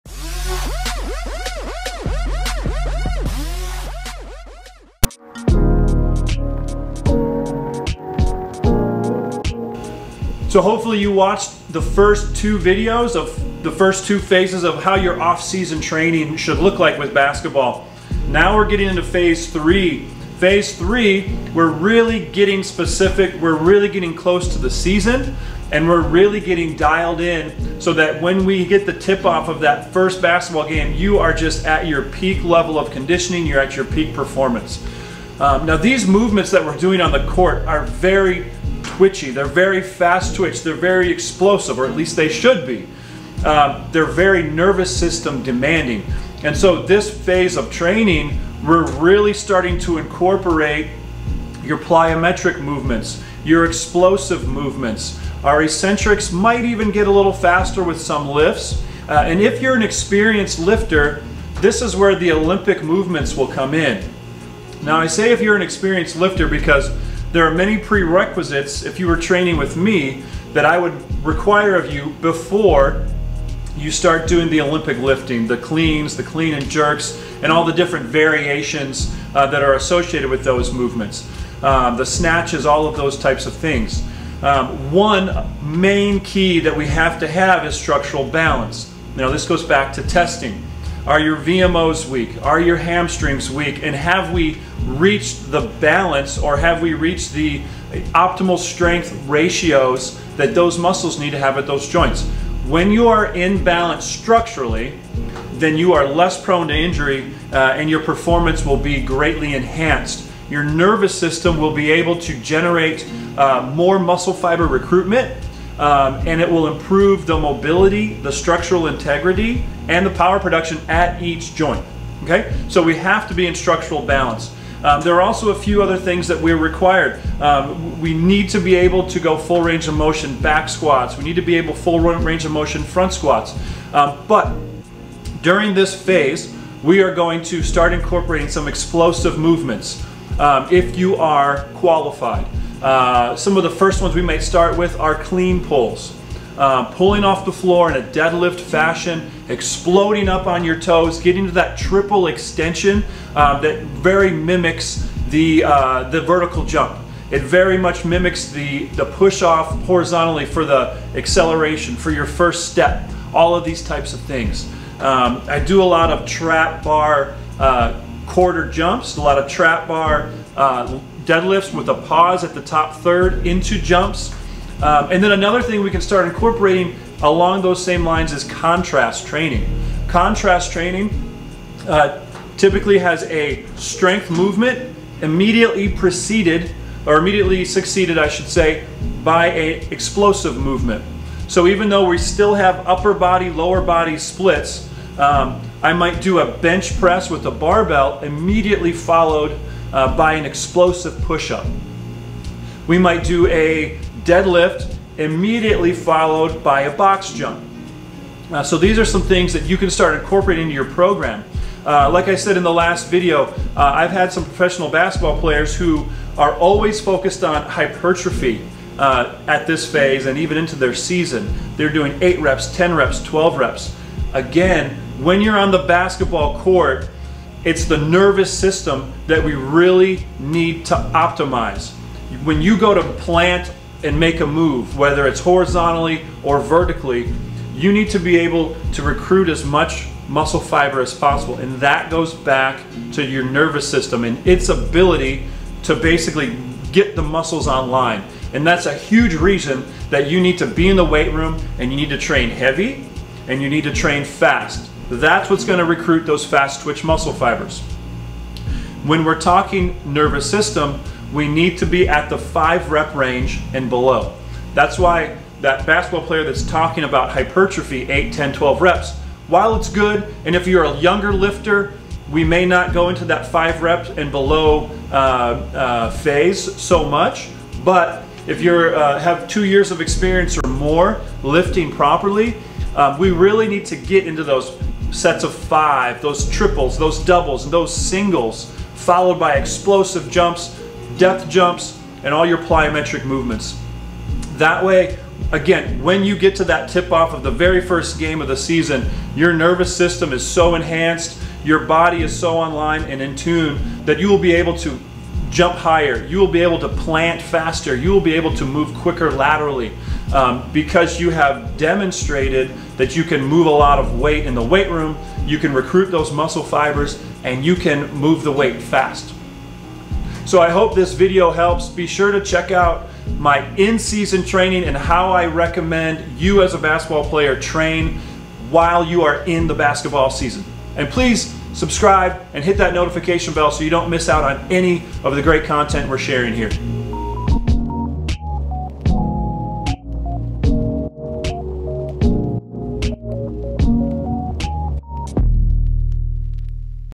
Offseason Basketball Training Program - Phase 3
If you want to peak your offseason training program just in time for the new season watch this video. I give you specific exercises to perform and strategies to use so you are your most explosive and quickest yet as you enter the new year.

You'll learn how to convert your strength into speed at exactly the right time - tip off of your new season. Here you'll learn plyometric moves, advanced training techniques and more, complete with video demonstrations.

In the first two videos of this series you learned exactly what you need to do to start your offseason training right. In this video I cover how to peak your training as you head into the coming season.